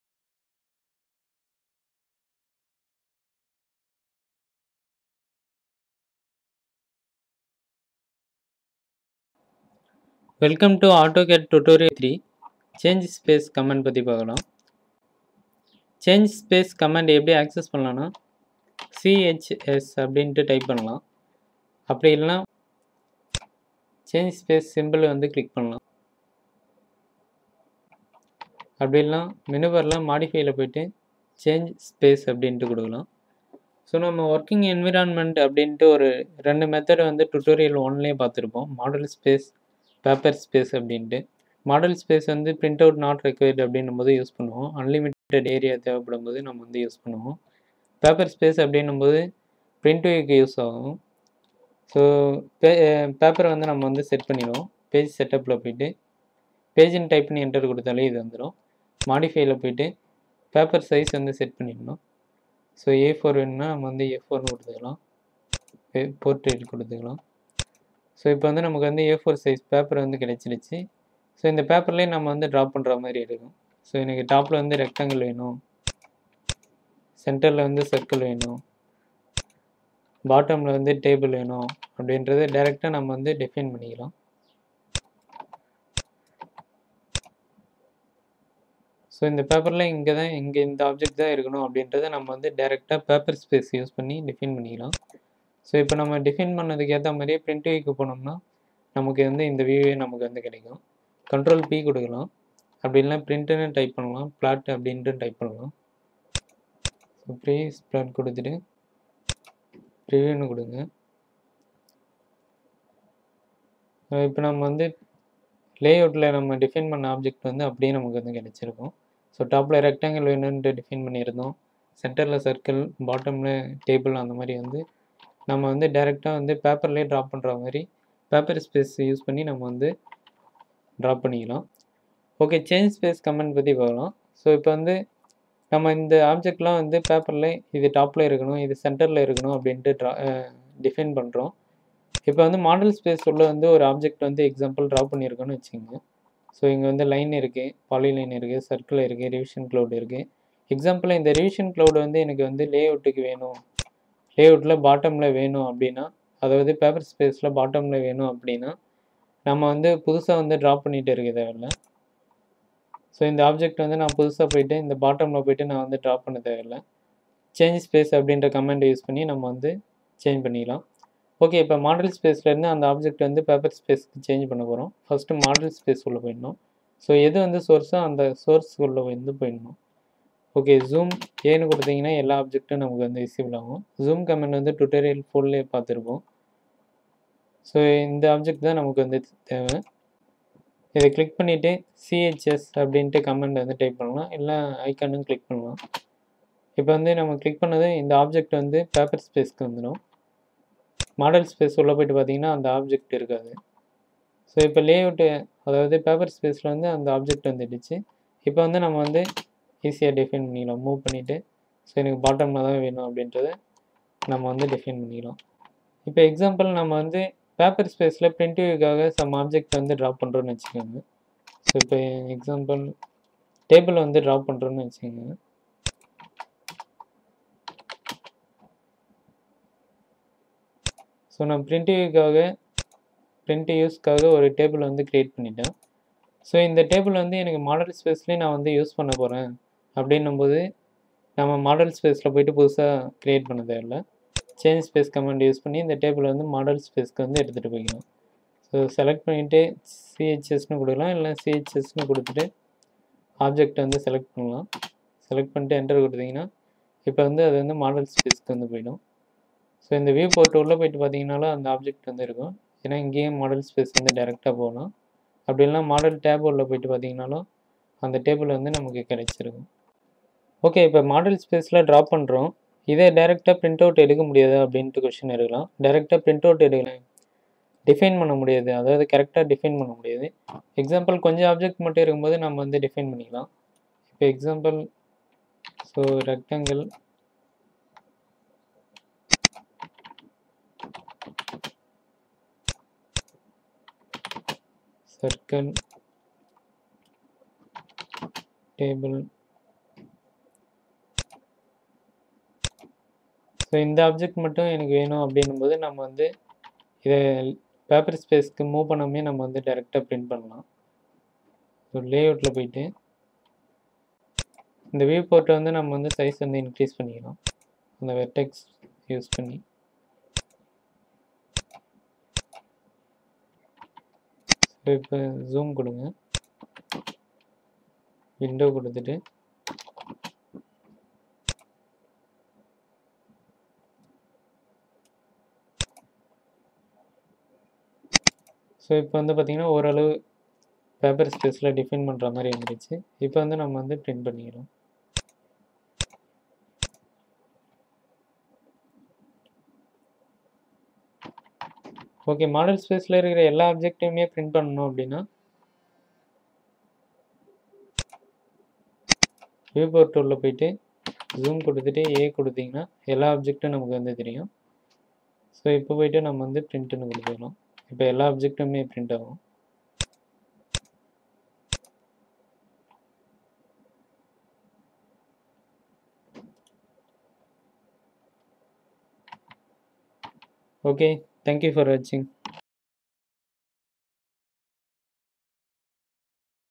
49 (MODIFY TOOL) CHANGE SPACE (CHS) COMMAND AUTOCAD, OBJECT CHANGING BETWEEN MODEL AND PAPER SPACE
HOW TO USE CHANGESPACE COMMAND EXPLAINED IN TAMIL